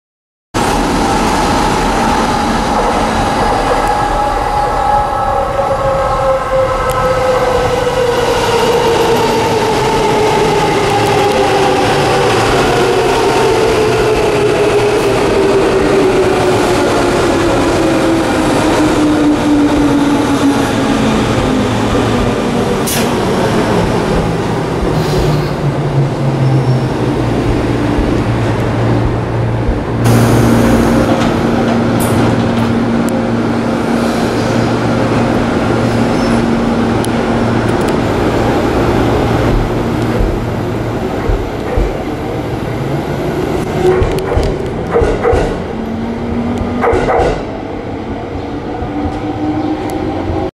埼京線205系ハエ24編成 各駅停車大宮行 与野本町駅にて
与野本町駅入線・発車する、埼京線205系の宮ハエ24編成。
この編成は2013年10月に運用離脱され、インドネシアJABOTABEK鉄道に譲渡されましたがもう既に現地到着済みのようです。日本でのご活躍に感謝致します。
2013年8月11日撮影